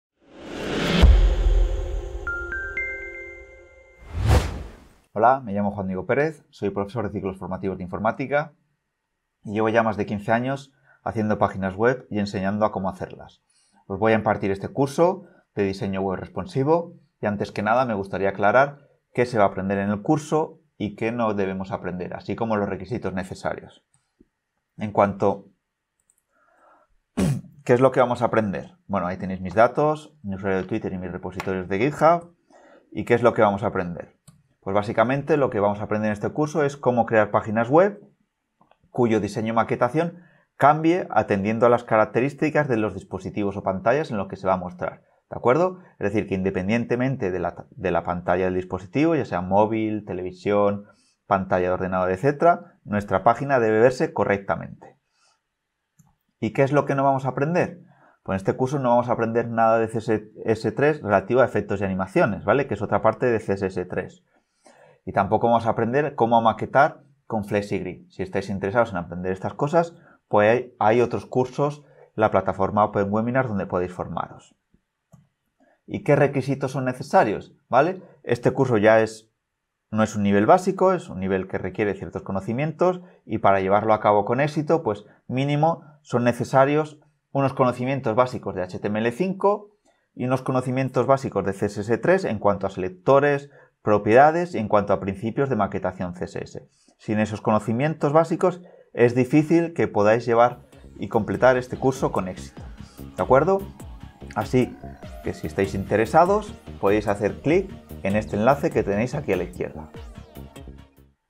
CURSO DE RESPONSIVE WEB DESIGN
Aprende a crear páginas Web responsive utilizando HTML y CSS, y empieza a realizar páginas Web profesionales que se adapten a todo tipo de dispositivos y navegadores ►► Suscríbete para seguir ampliando tus conocimientos